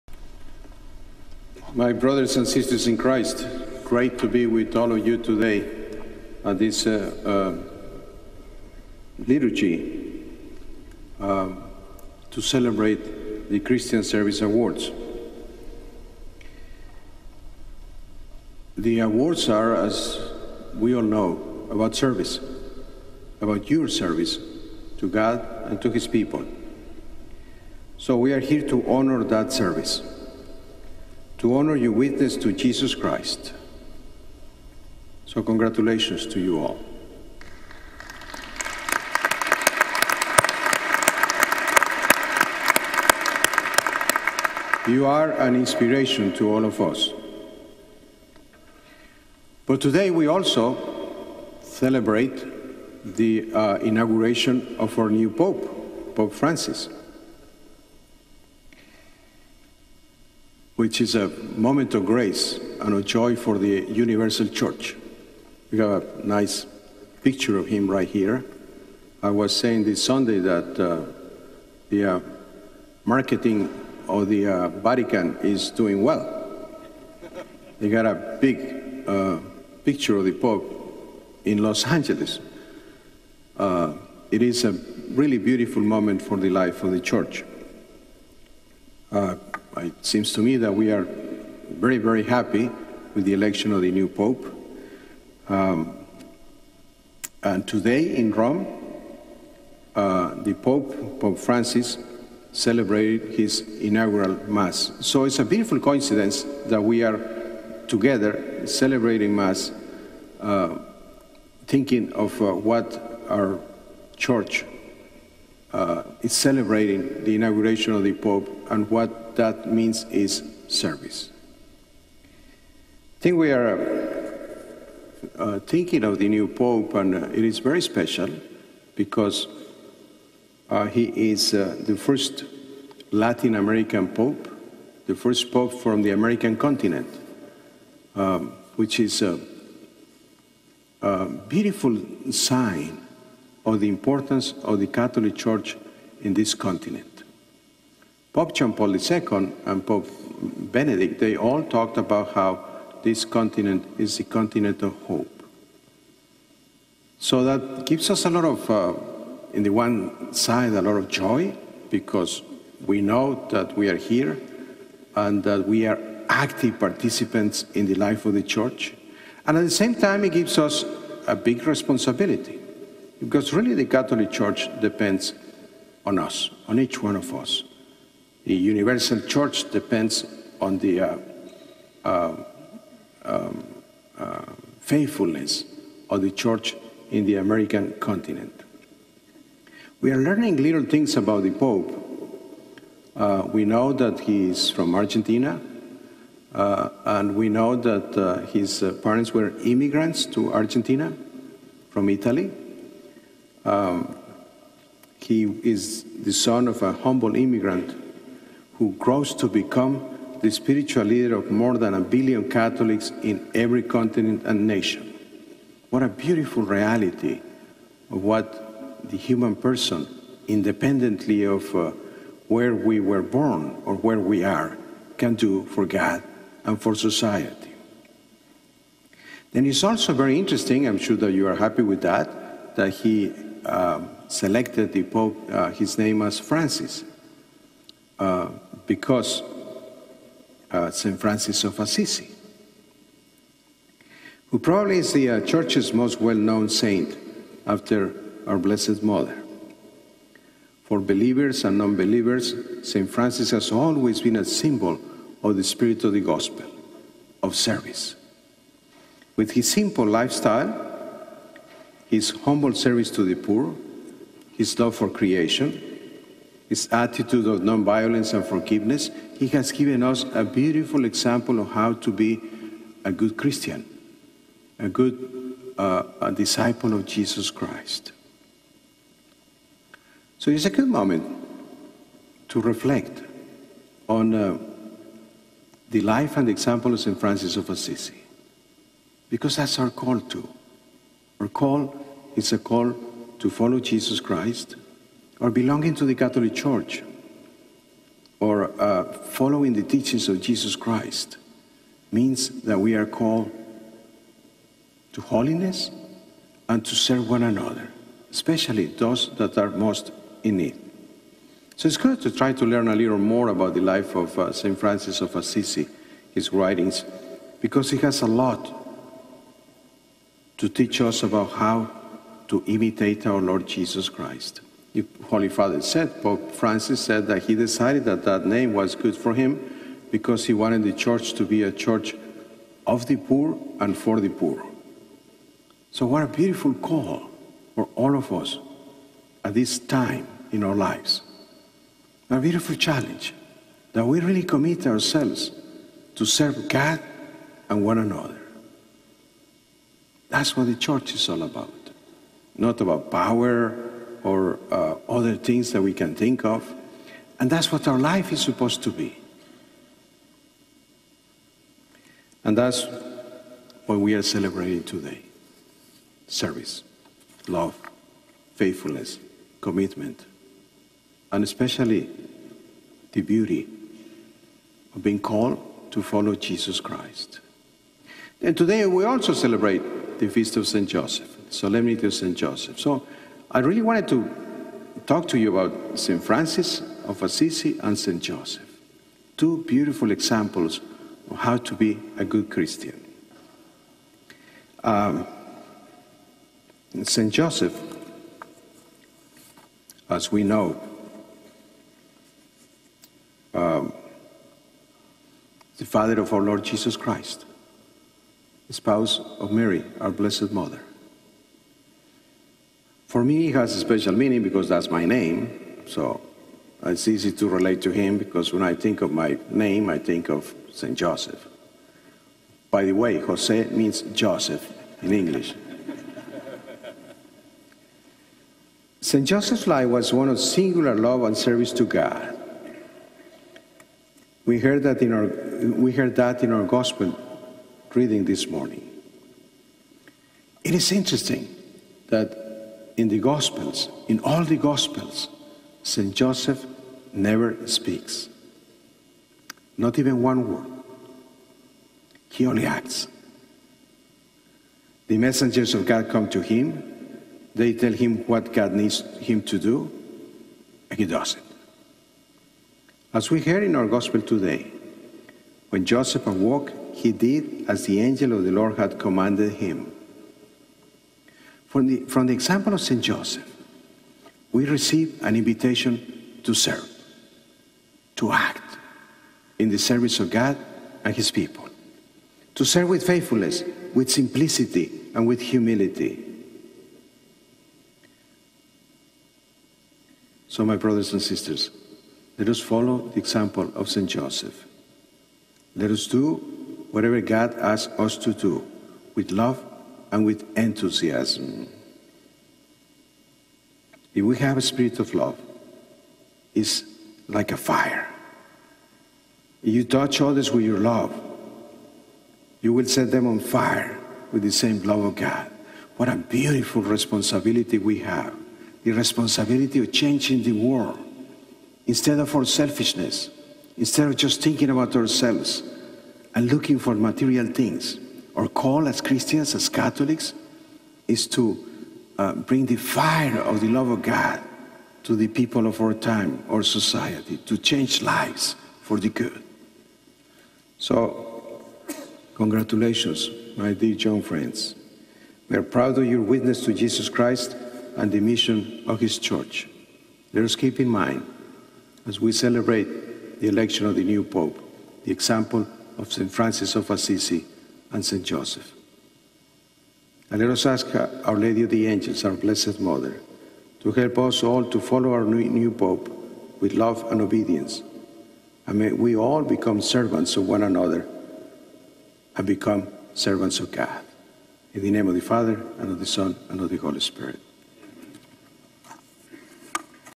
Homily: Christian Service Awards / Mass for the inaguration of Pope Francis (3/19/2013)
On Tuesday, March 19, 2013, Archbishop Gomez celebrated the Archdiocesan Christian Service Awards Mass at the Cathedral. The Mass also celebrated the inauguration of the Holy Father, Pope Francis, which took place hours earlier in St. Peter's Square in Rome.  Archbishop Gomez spoke how it was fitting that the Christian Service Awards Mass was being held today, especially that the Holy Father's message today focused on service and looking after the poor.